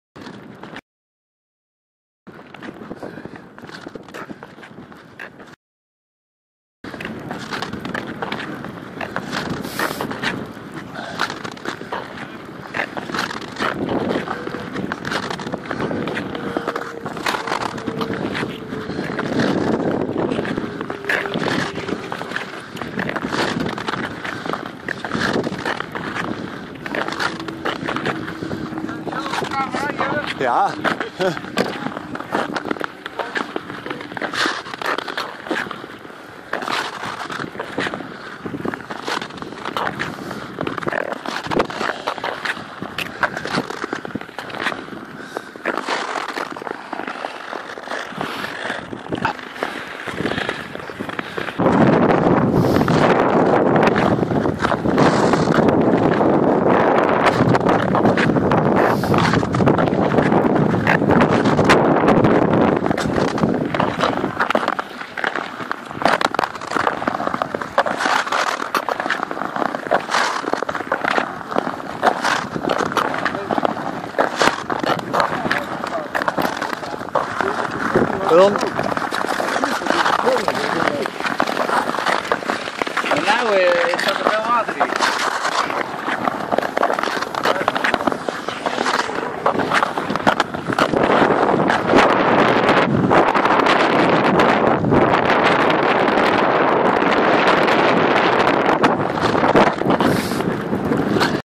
Schaatsen op het Quackjeswater
Op de geveegde baan op het Quackjeswater.
Zaterdagmorgen 26 januari rond 11;30uur.